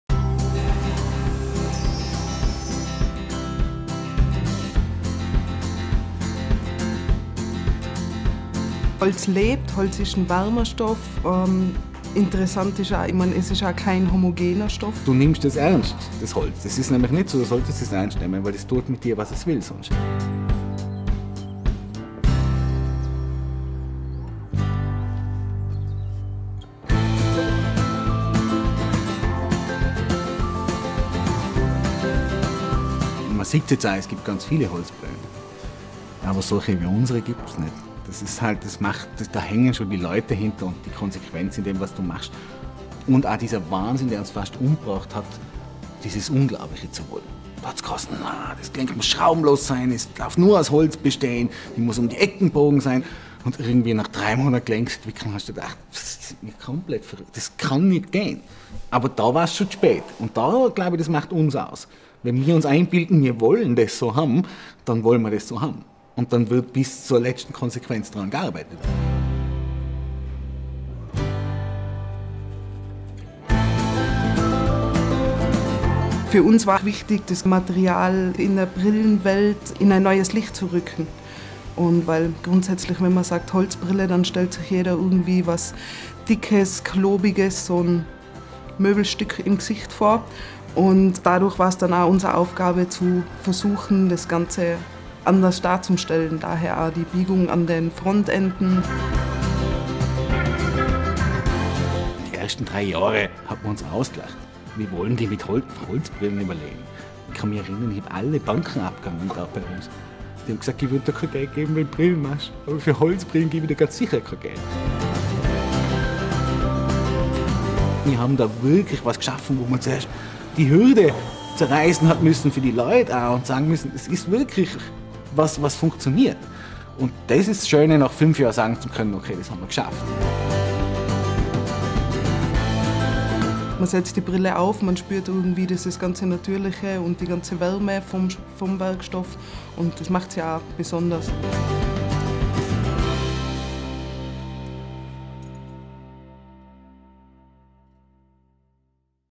Was macht eine ROLF Holzbrille besonders und wie werden die Brillen hergestellt?
„Du solltest das Holz ernst nehmen. Es tut sonst mit Dir, was es will“

Ja, Roland Wolf hat einen Brillentick, keine Frage. „Mit 14 Jahren habe ich angefangen, Brille zu tragen, obwohl ich gar keine brauch“, gesteht er. Es ist genau diese Begeisterung, mit der es der Tiroler geschafft hat, seine Firma ROLF Spectacles zu einer der angesagtesten Brillenschmieden überhaupt zu machen. Wobei Schmiede eigentlich das völlig falsche Wort ist, denn die Brillen aus Tirol bestehen komplett aus Holz.

Im Video wird gezeigt wie und wer die ROLF Holzbrillen, Steinbrillen und Hornbrillen in Handarbeit hergestellt werden.

© Initiative Furnier + Natur e.V.